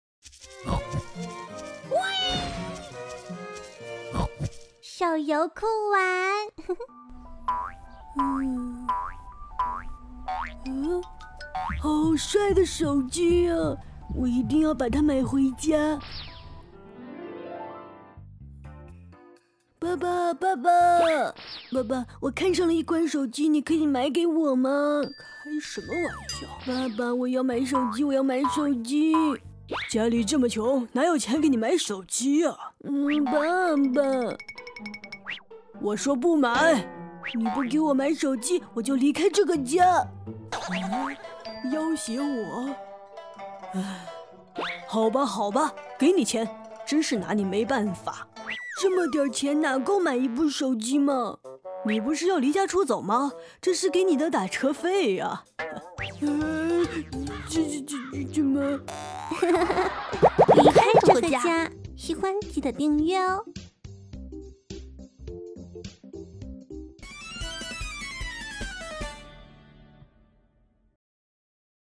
【植物大战僵尸同人动画】离开这个家-游戏搞笑动画
小坚果以离家出走要挟爸爸给他买一部手机，果然，爱着小坚果的爸爸拿出了钱……
剧情的发展真的有那么简单吗？快来看看吧！